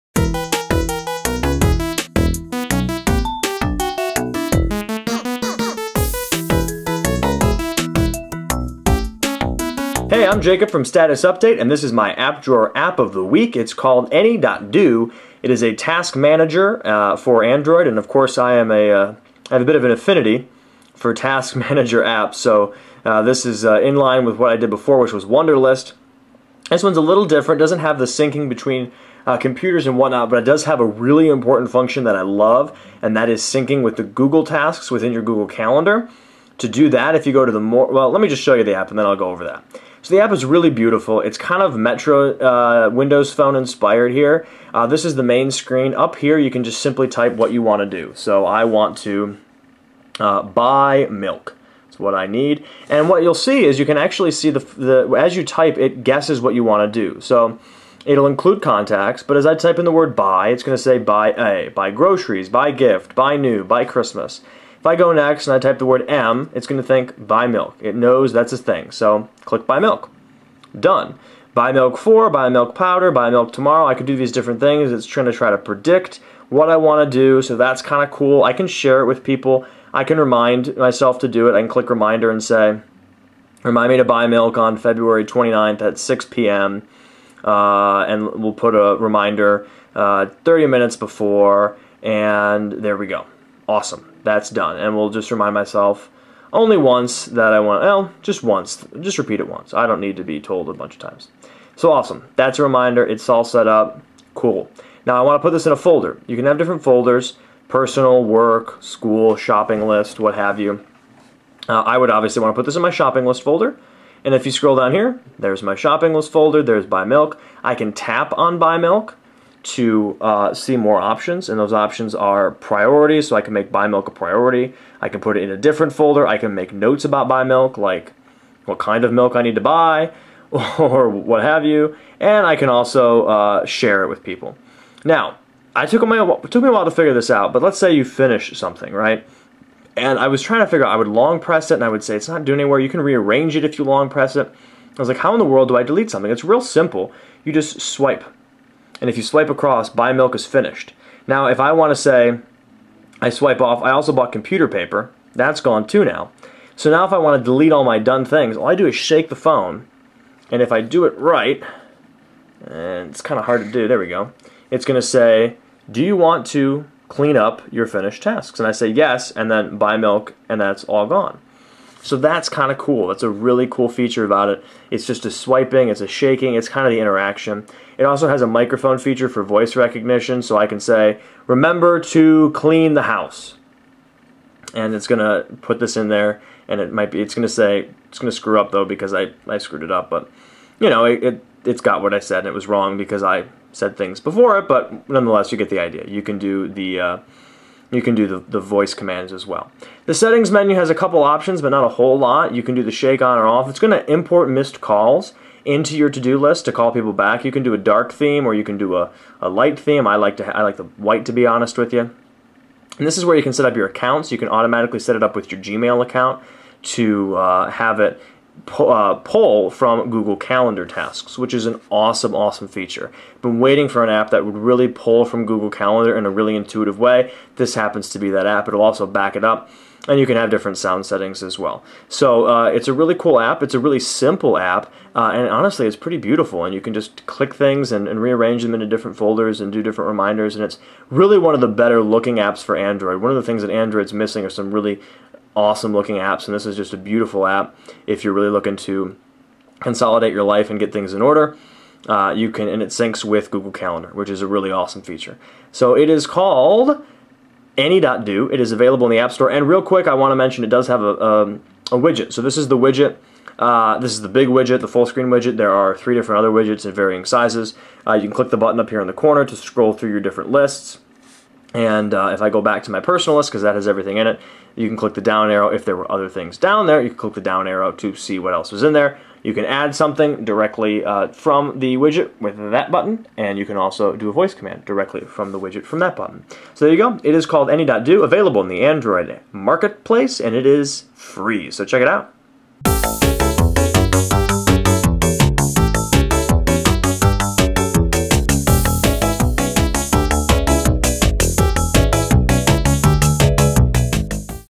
App Drawer - Any.Do
Jacob takes a look at a beautiful tasks app Any.Do.

With this app you can organize your to-do list in a beautiful way, you can set reminders, organize into folders, and even sync with the tasks pane of your Google Calendar.

Status Update is a social media and social tech podcast, every Sunday at 7pm EST